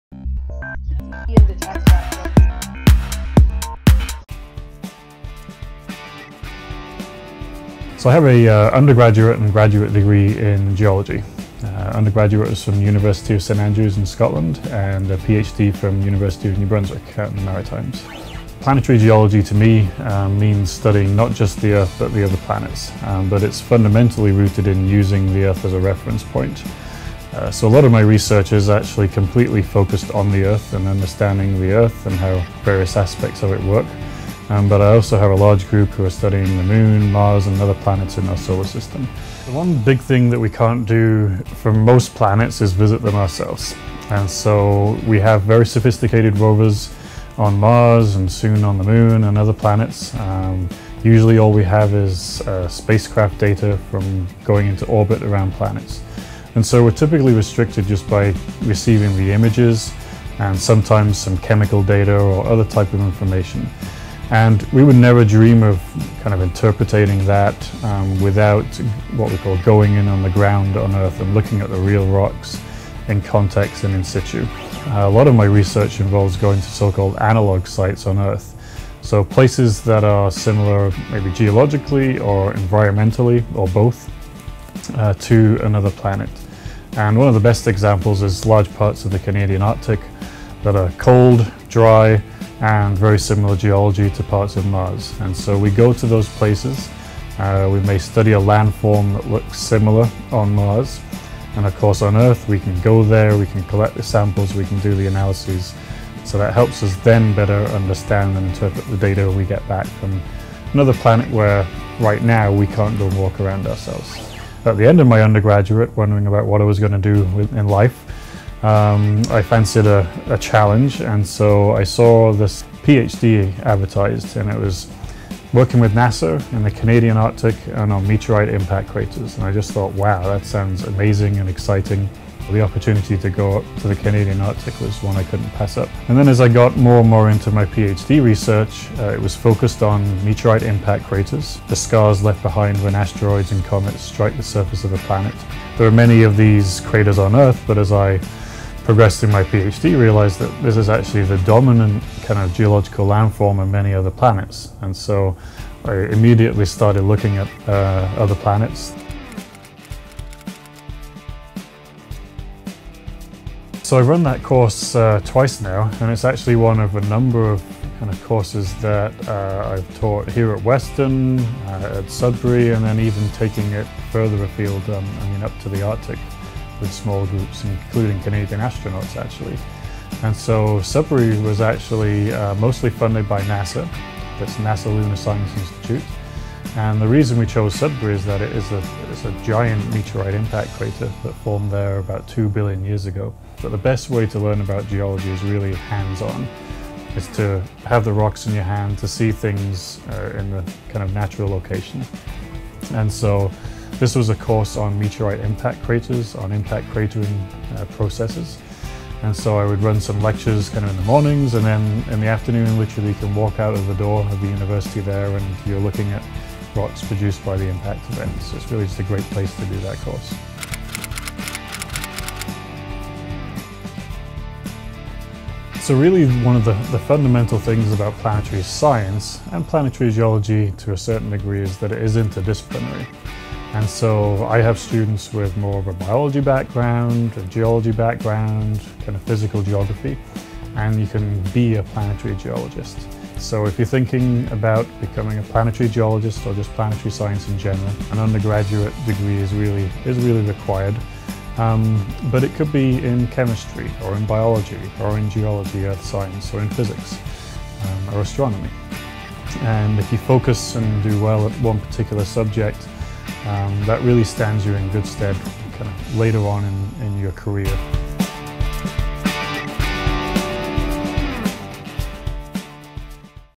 Science Scoop - Planetary Geology at Western University
What is planetary geology? Planetary geology is the study of the other planets within our solar system. Dr. Gordon Osinski from Western University is here to talk about planetary geology as well as his research.